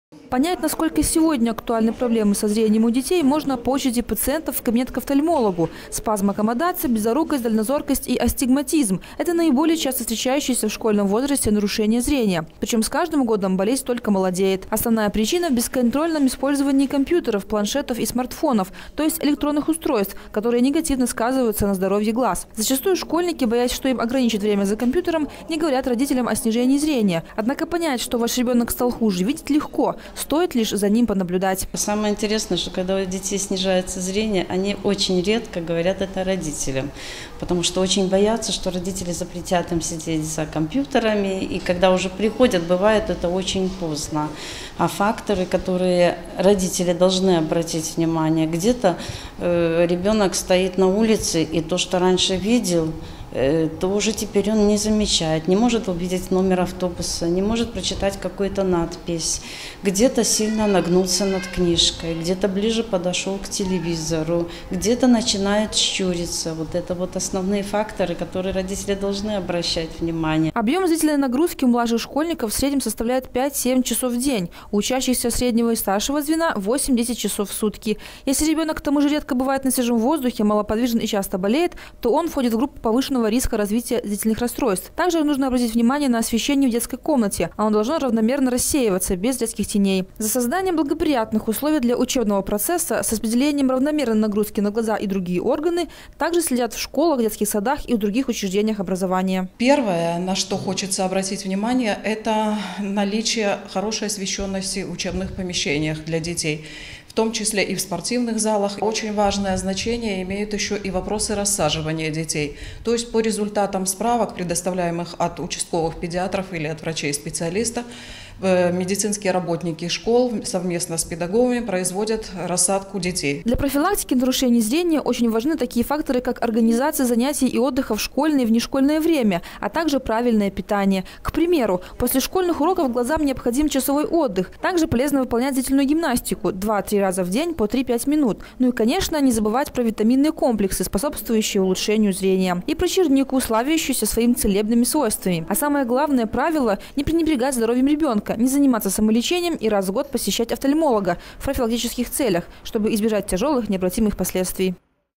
2015-09-10 г. Брест. Как не навредить зрению в погоне за знаниями. Телекомпания Буг-ТВ.
Как не навредить зрению в погоне за знаниями – советы офтальмолога.
Совсем недавно у школьников Брестчины начался новый учебный год. Книги, тетради, домашние задания – очередной сезон знаний   скучать явно не даст. В то же время учебный процесс связан со значительным увеличением нагрузки на весь организм юных берестейцев, и, в частности, на глаза.
По данным белорусских медиков, различные болезни зрения  имеют почти 70% школьников. Как не допустить появления и развития офтальмологических проблем смотрите в сюжете